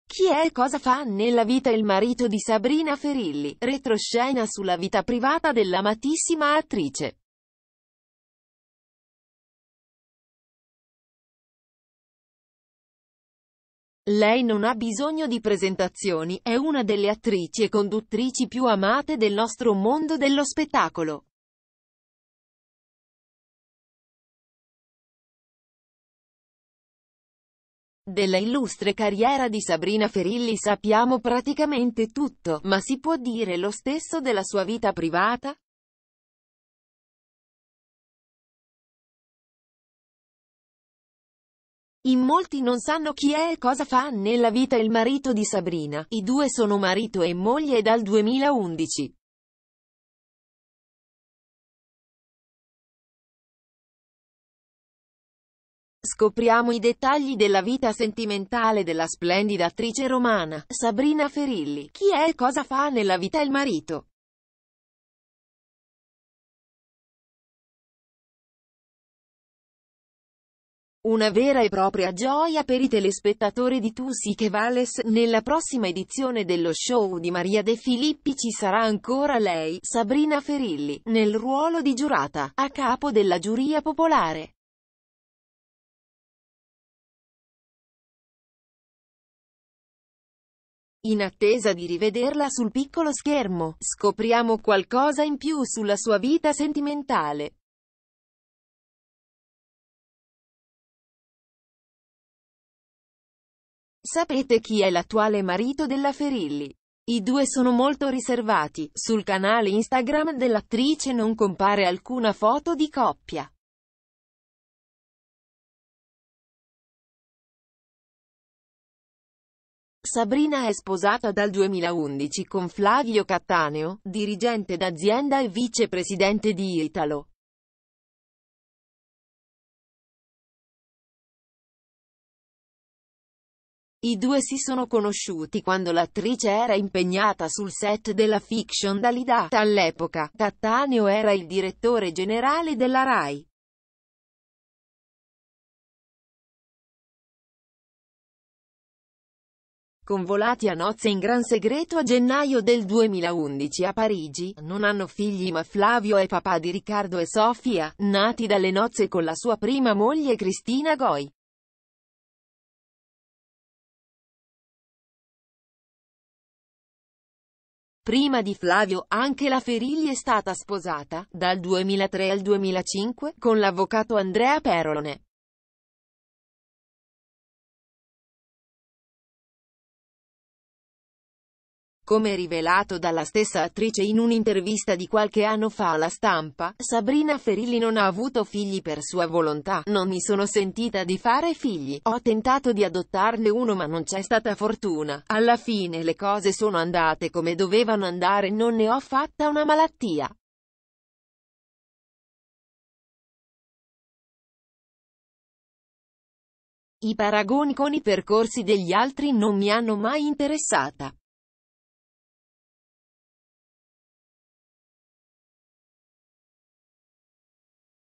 Chi è e cosa fa nella vita il marito di Sabrina Ferilli retroscena sulla sua vita privata
Chi è e cosa fa nella vita il marito di Sabrina Ferilli: retroscena sulla vita privata dell’amatissima attrice.